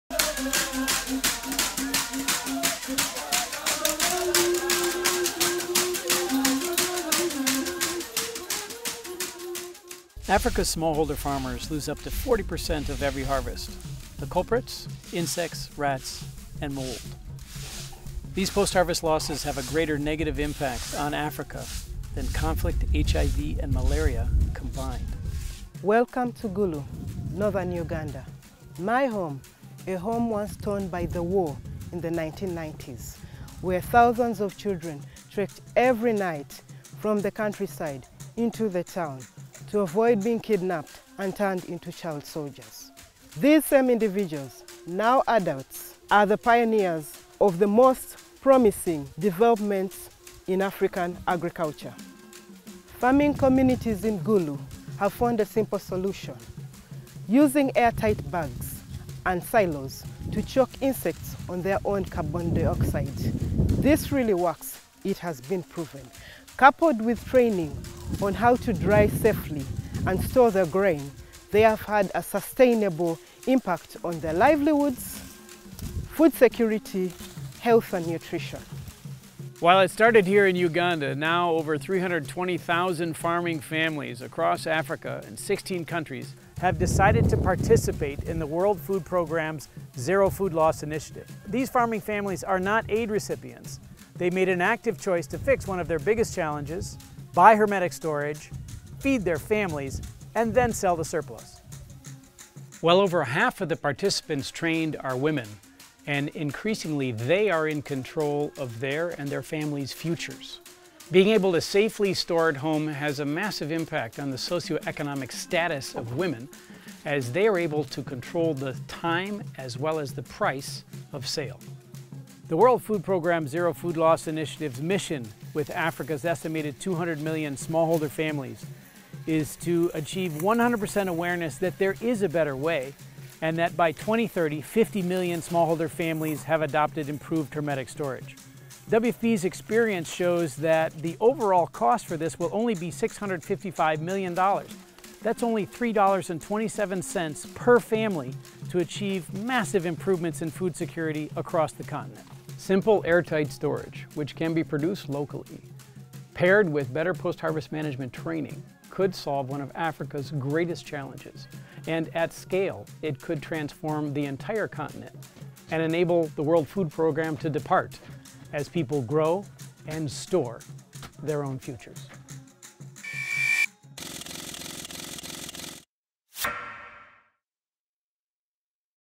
WFP’s Zero Food Loss Initiative: reaching 200 million farmers by 2030
Africa’s smallholder families lose up to 40 percent of their harvest to insects, rodents and mould. But a simple solution, airtight storage bags and silos – combined with training on how to safely dry their grain – can virtually eliminate such losses and improve families’ incomes, food availability, health and nutrition. WFP’s Zero Food Loss mission is to reach all of Africa’s 200 million smallholder families so they are aware there is a better way: one that could transform the continent and solve one of Africa’s biggest challenges. And that at least 50 million families acquire air-tight silos or bags by 2030. WFP’s experience shows that it will cost only 655 million dollars over the next 10 years to reach these two objectives. That’s only $3.27 per family – to achieve drastically improved food security across the continent. Special thanks to Ndere Foundation for allowing WFP to use its Larakaraka music